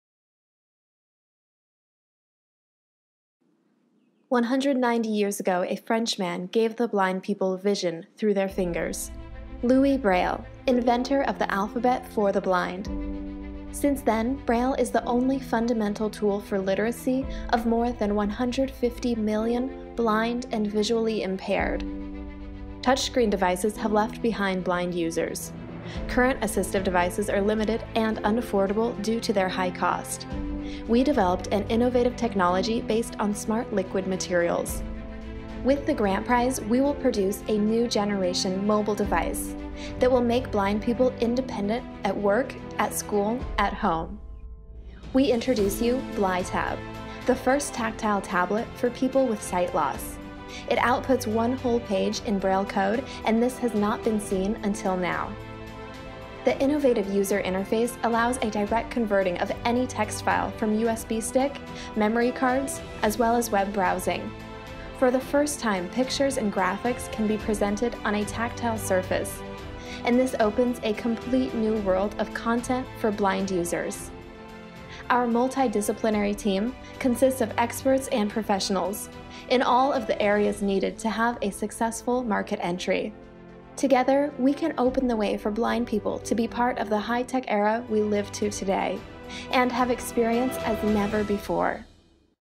Innovative Practice 2016: BLITAB — First Tactile Tablet for Blind People
Beginning three years ago in Bulgaria, BLITAB® Technology developed the first-ever Braille tablet, using a new liquid-based technology to create tactile relief that produces Braille, graphics, maps, etc., for the blind and partially sighted. From the very start, blind and visually impaired users were involved in the product development because BLITAB is fully designed according to their needs, not adapted as many assistive devices are. It is called BLITAB® – the iPad for the blind, and for the first time it makes it possible for children and students with sight loss to finally have access to the latest technology and thus to learn and study more efficiently.